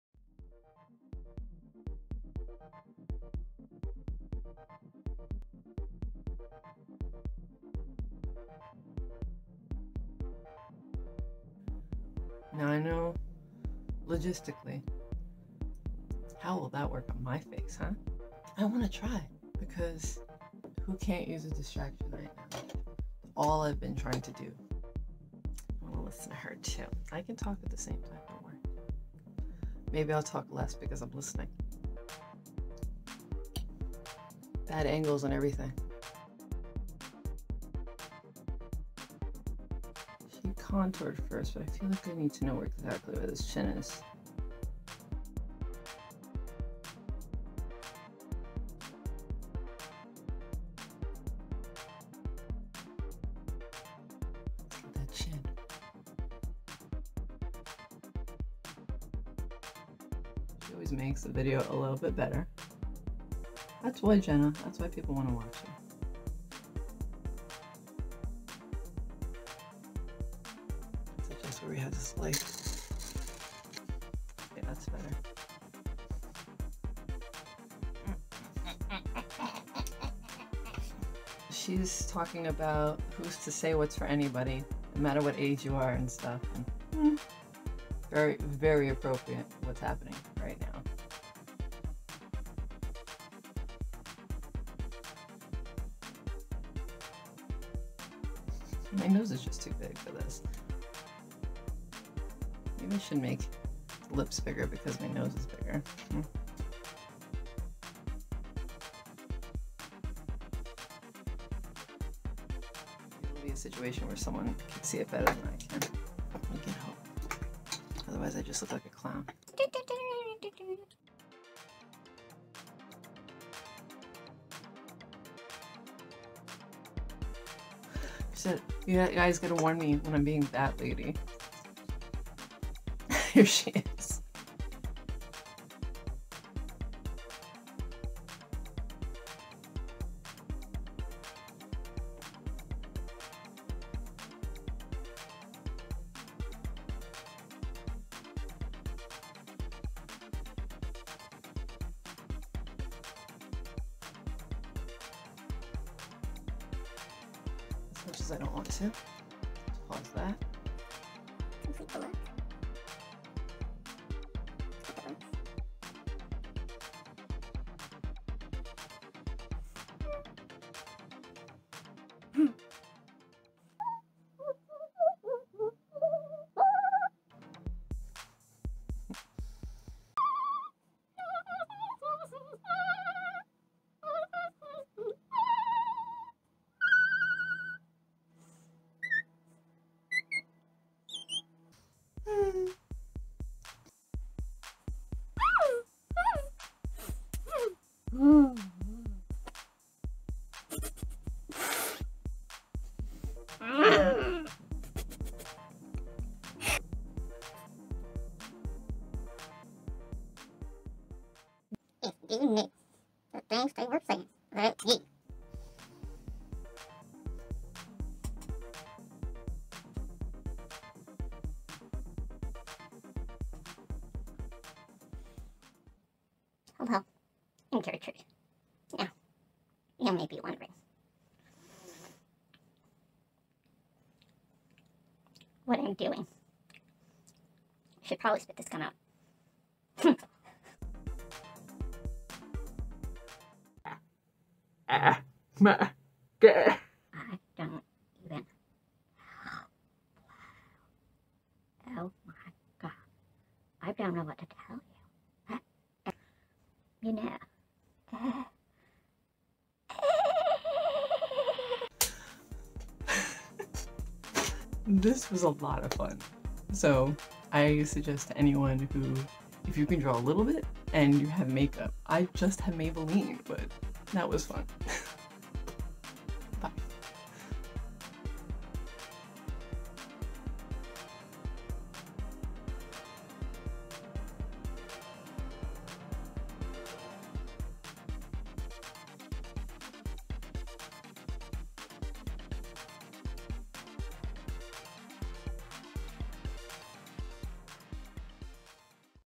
tiny face makeup
First, credit to all the amazing artists who've done this prior (there are so many looks tagged on Instagram):

It's been a long time since I saw a fun thing like this that could be a full challenge. I wanted to try it so I friggin' pushed myself to do it.

I only have drugstore makeup, and mostly Maybelline, but I pulled this off enough to entertain myself. :)